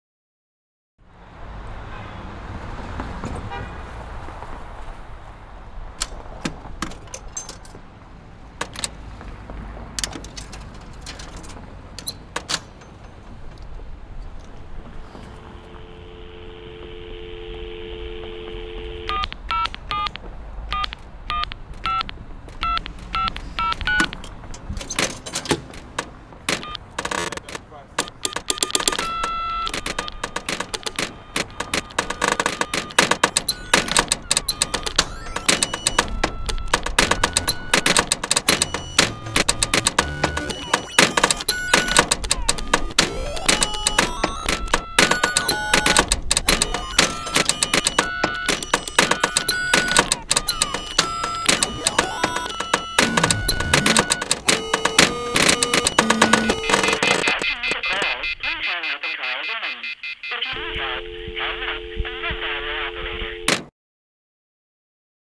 "Signal Display"
Tim Kling and Chris Harbach spent a weekend in downtown Orlando to make this short art piece. Tim shot all the photos, which are have not been altered digitally in any way. Chris carried around a laptop and a microphone, and gathered only "telephone" sounds which make up 100% of the soundtrack in this piece.